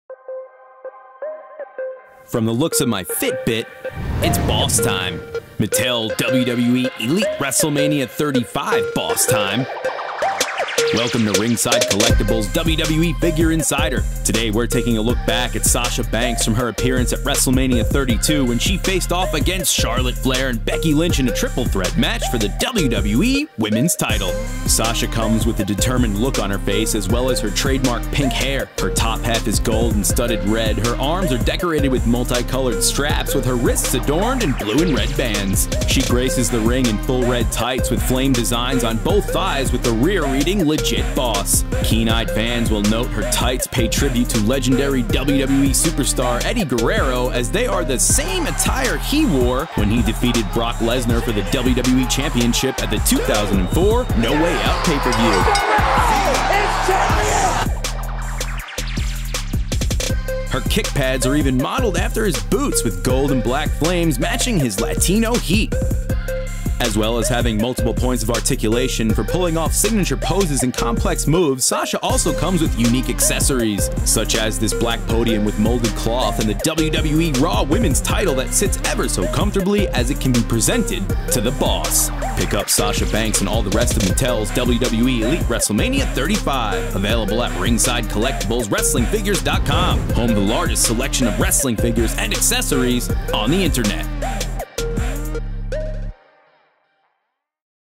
WWE FIGURE INSIDER: Sasha Banks - WWE Elite WrestleMania 35
Everyone knows a Legit Boss is the real deal with the Mattel WWE WrestleMania 35 Elite Sasha Banks! Banks has a new headsculpt, removable glasses and molded chain. She's wearing a gold and red designed ring top and colorful arm and wrist bands. Her red ring tights have a burst of color designs on the sides and she has black and gold 'Sasha Banks' boots! She also includes the WWE RAW Women's Championship and a black stand to display it!

Sasha Banks - WWE Elite "WrestleMania 35" WWE Toy Wrestling Action Figure by Mattel!

The Boss, Charlotte Flair, and Becky Lynch proved why they defined the women's Revolution at WrestleMania 32.

Career Highlights:

RAW Womens's Champion

NXT Women's Champion